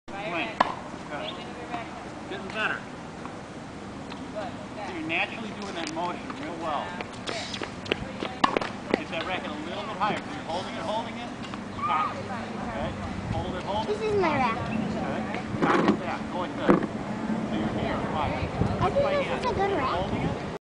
Jake Takes a Tennis Lesson III
Jake takes a tennis lesson from Rick Wessig at Mission Hills.